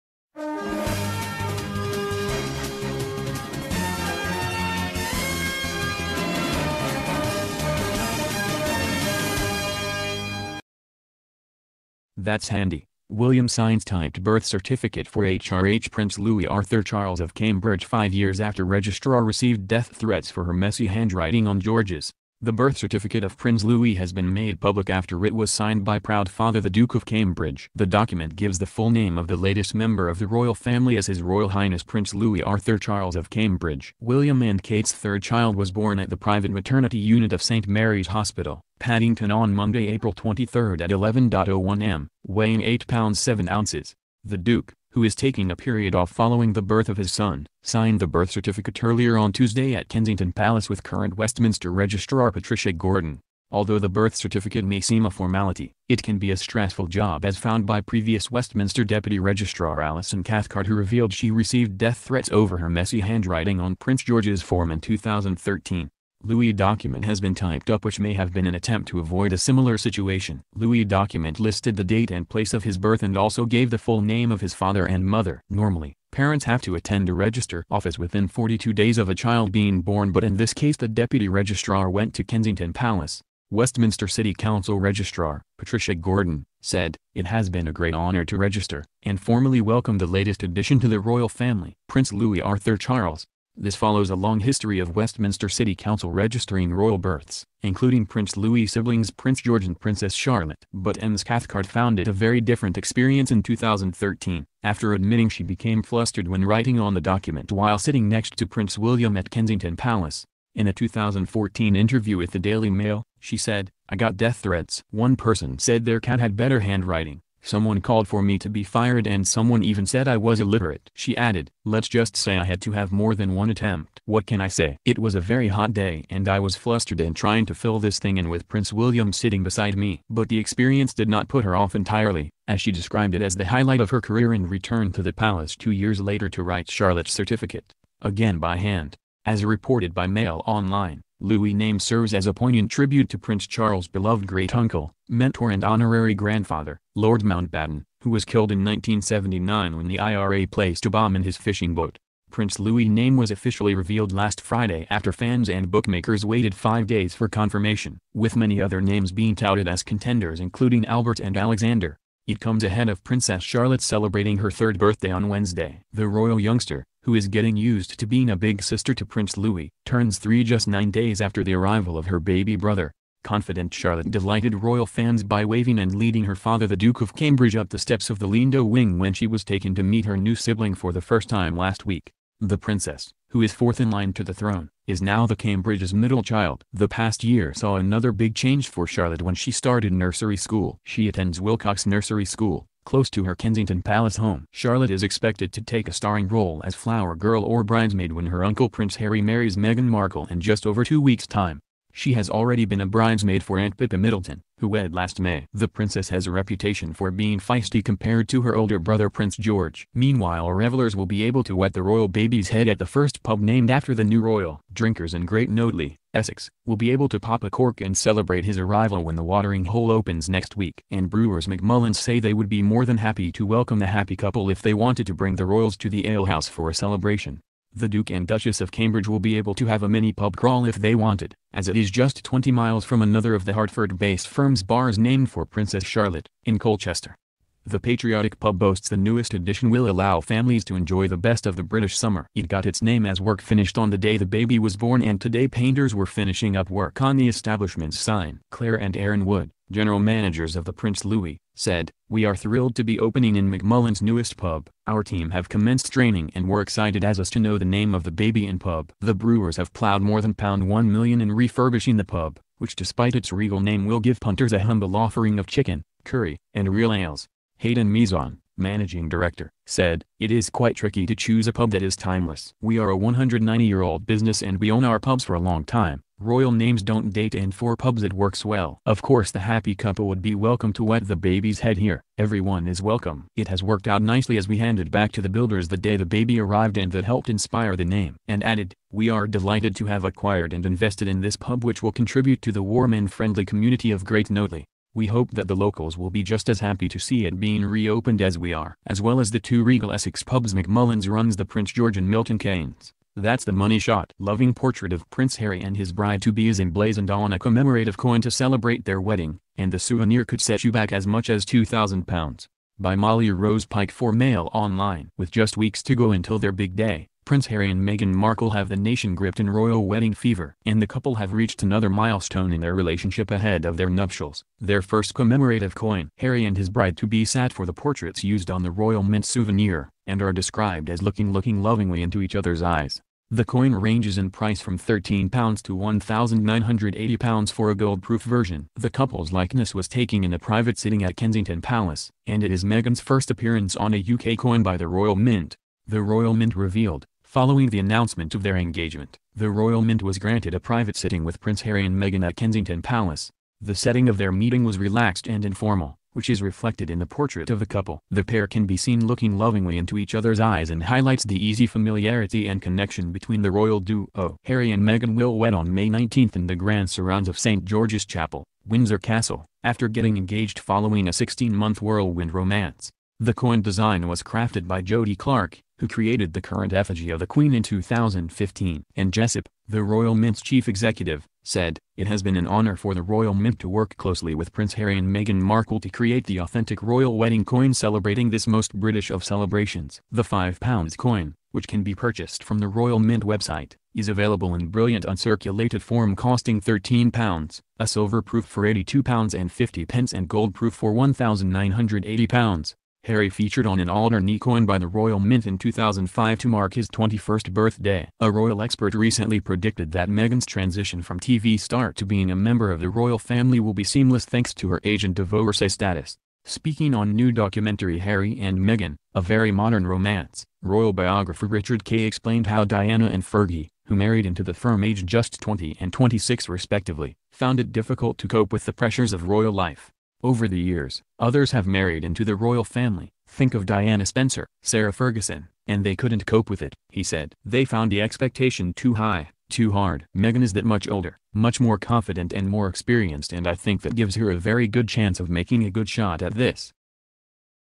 Prince William and Kate Middleton released Prince Louis' official birth certificate this morning
That's handy! William signs TYPED birth certificate for HRH Prince Louis Arthur Charles of Cambridge five years after registrar received DEATH THREATS for her messy handwriting on George's.